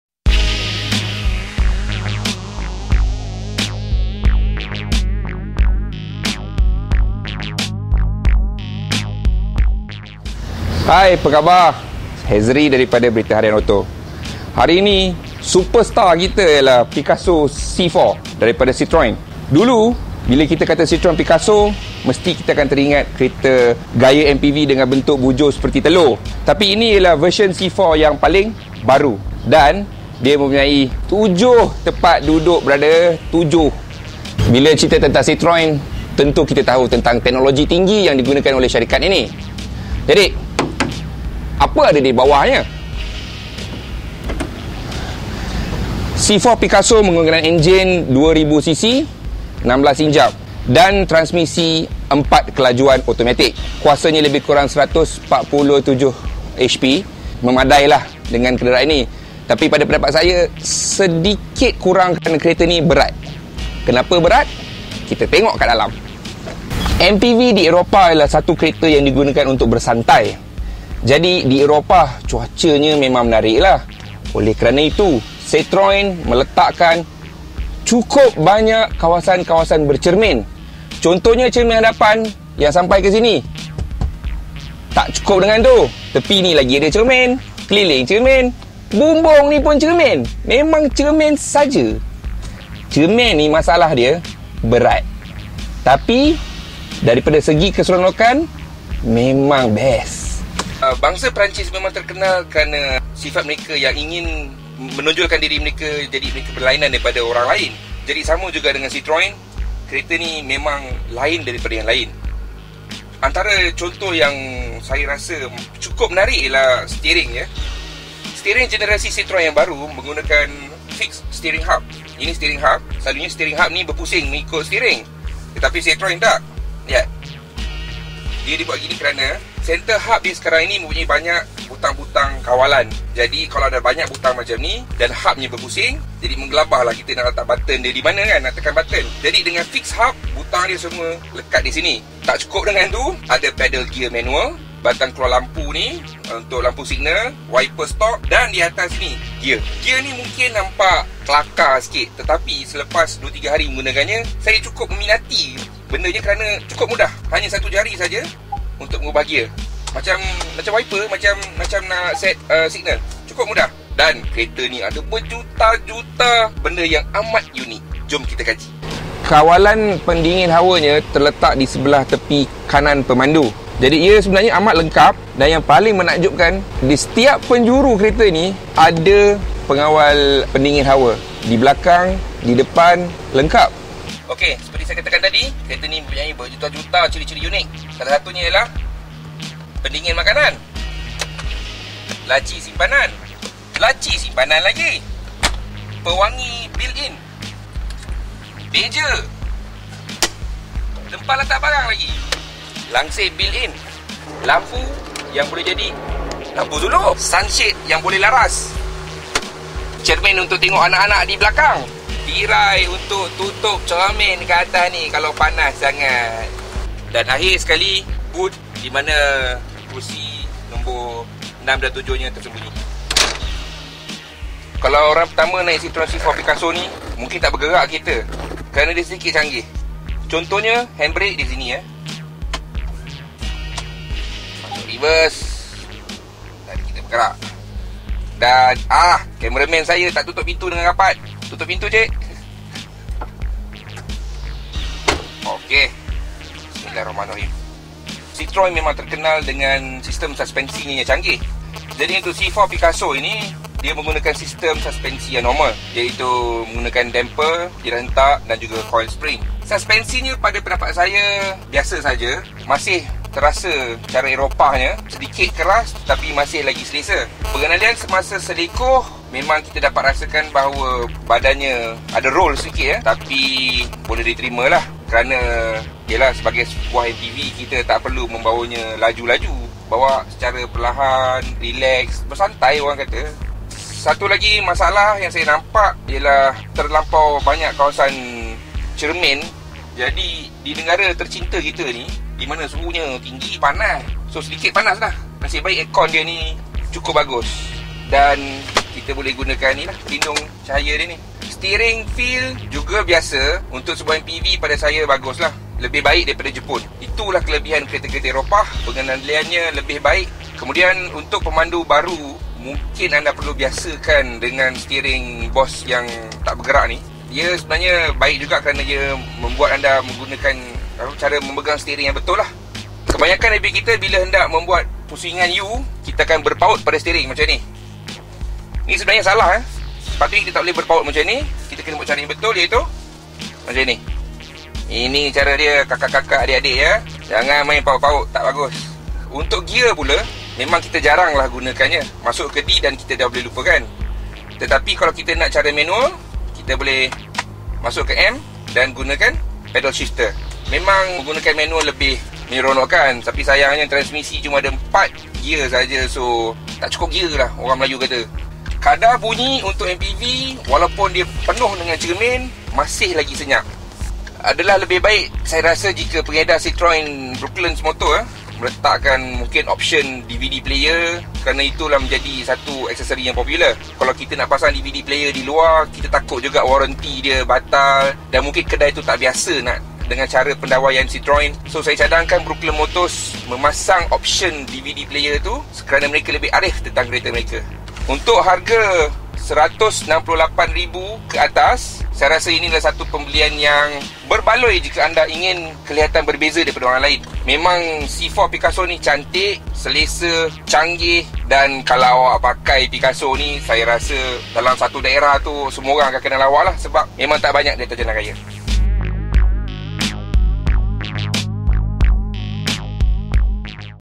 BHauto - Citroen C4 Picasso Terbaru
(Malay) Hezeri Samsuri cuba memahami betapa indahnya sebuah ciptaan bernama C4 Picasso.